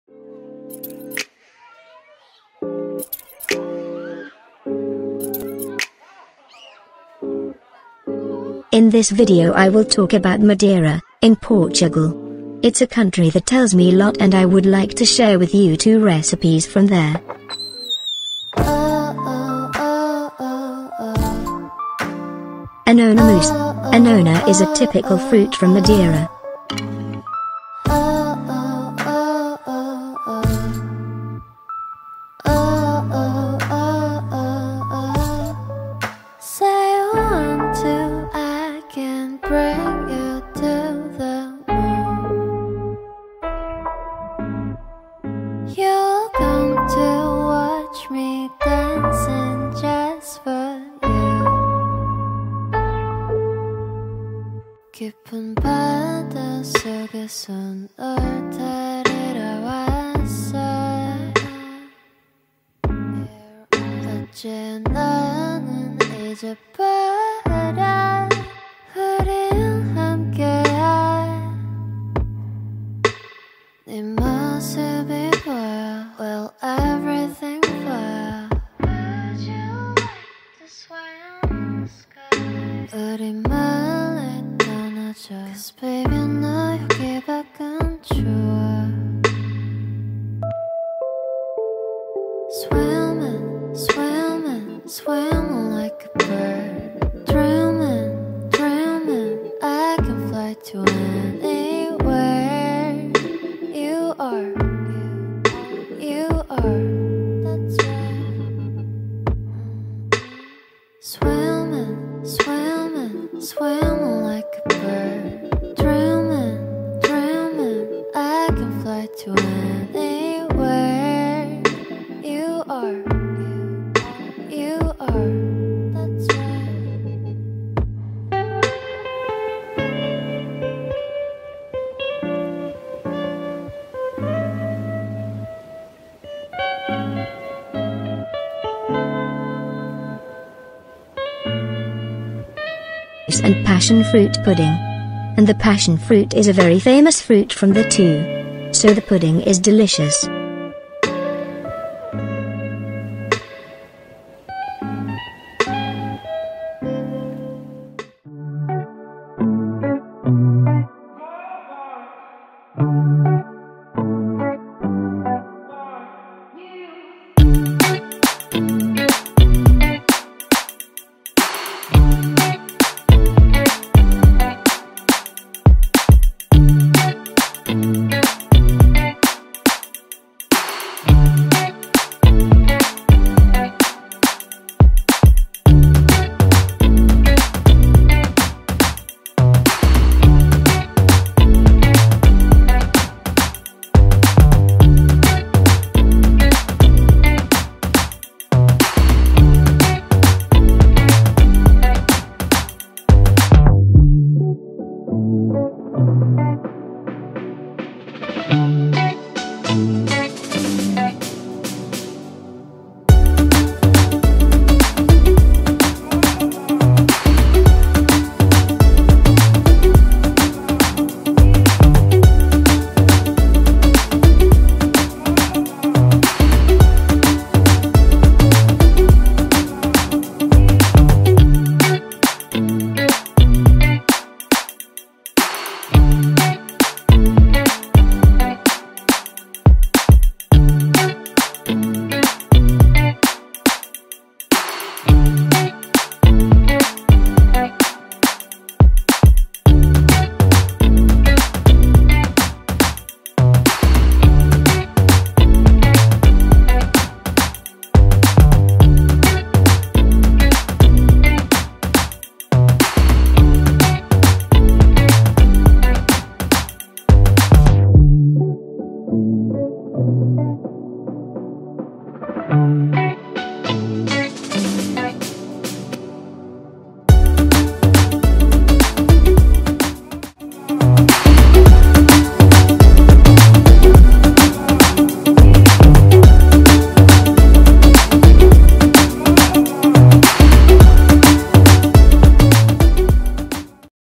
Making Desserts from Madeira, Portugal (Anona Mousse + Passion Fruit Pudding)
Hey everyone! This week we have a video to make your mouth water! Two delicious recipes from Madeira, in Portugal.

Anona Mousse
- 4 anonas
-2 cream packages
-3 gelatin sheets
-20g sugar (optionally)

Passion Fruit Pudding
-420g passion fruit pulp
-150 ml milk
-5 gelatin sheets
-2 cream packagess
-1 tin of condensed milk

Syrup (Of Passion Fruit Pudding)
-100g passion fruit pulp
-1 package pineapple gelatin

I bet you will love it! It´s perfect to impress your family now, at Easter.

Stay safe and well

2 You